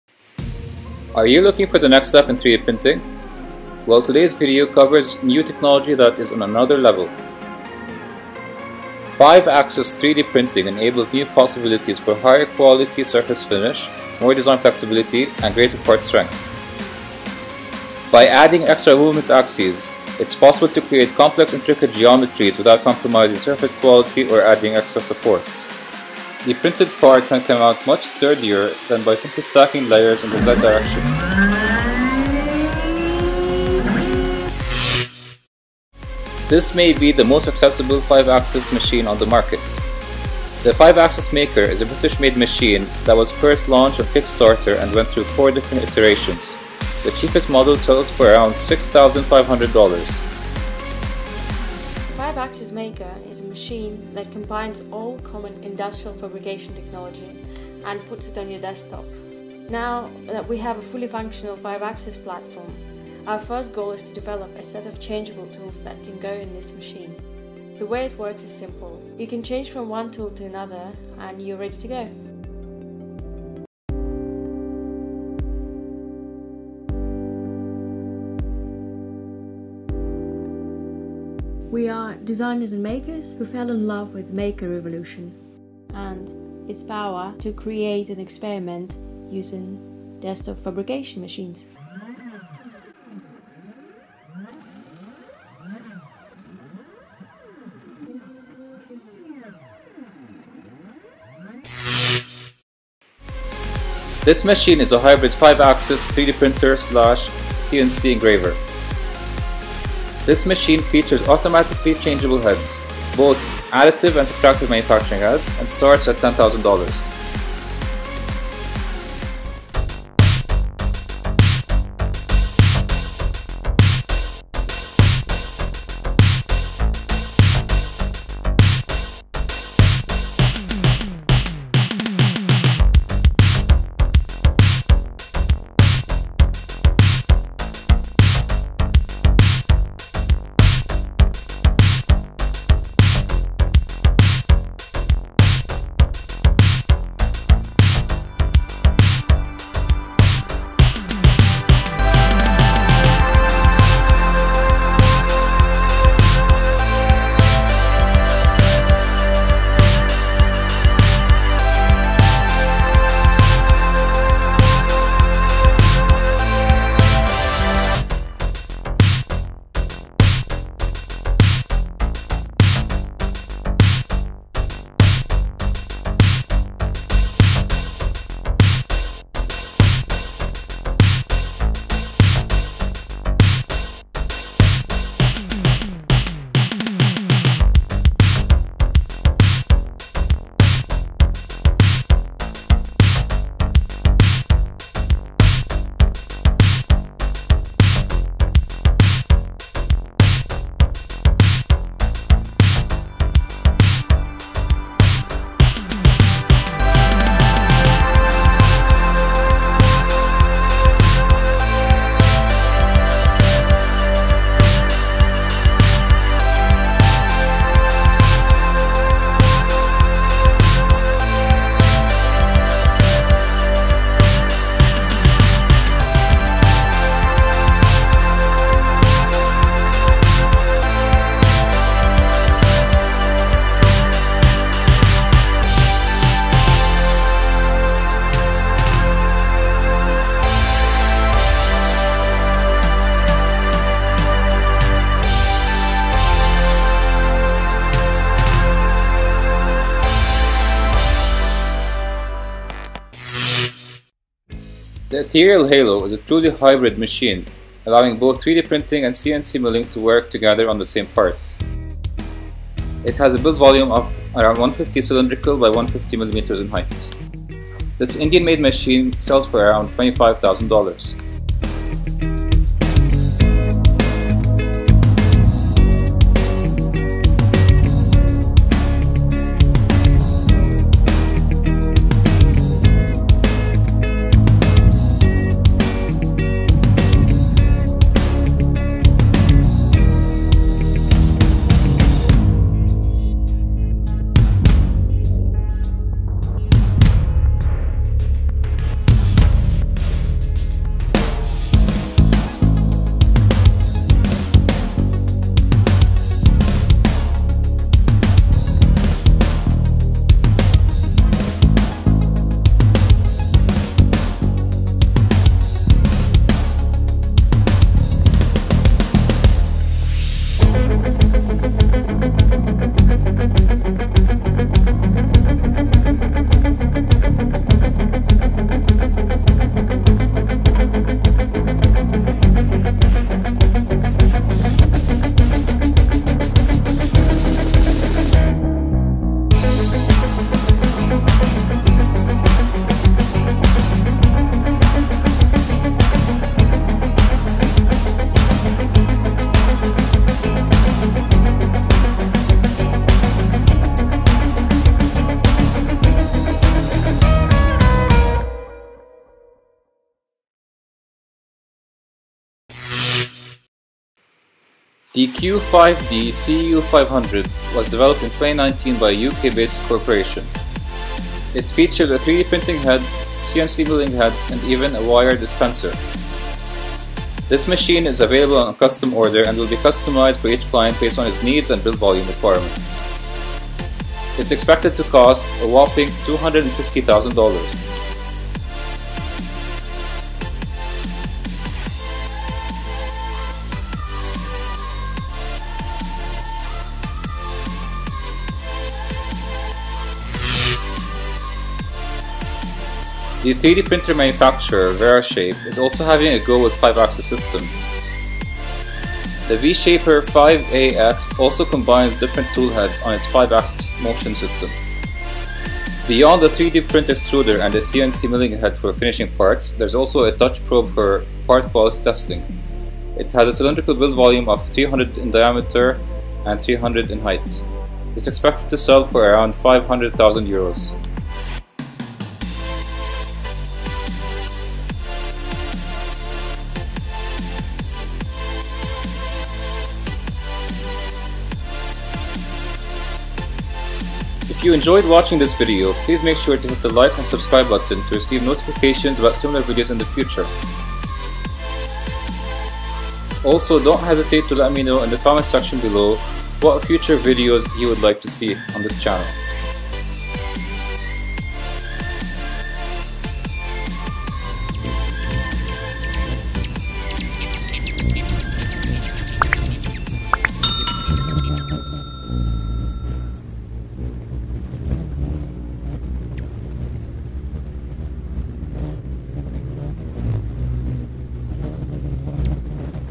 3D printers that can print with 5-axis Control | The 5 Best 5 Axis 3D printers in 2021
The top 5 3D printers as of 2021 with 5-AXIS control